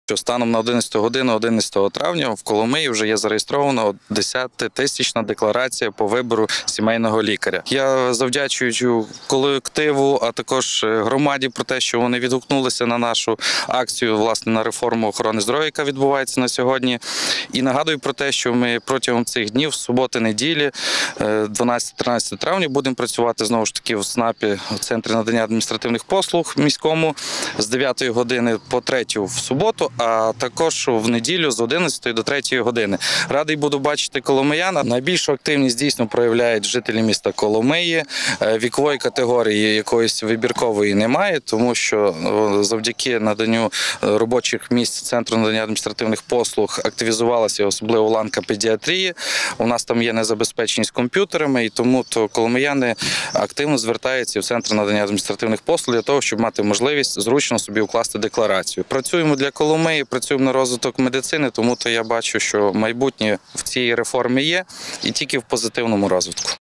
12-13 травня у Коломийському ЦНАПі можна буде обрати сімейного лікаря
12-13 травня у Коломийському ЦНАПі можна буде обрати сімейного лікаря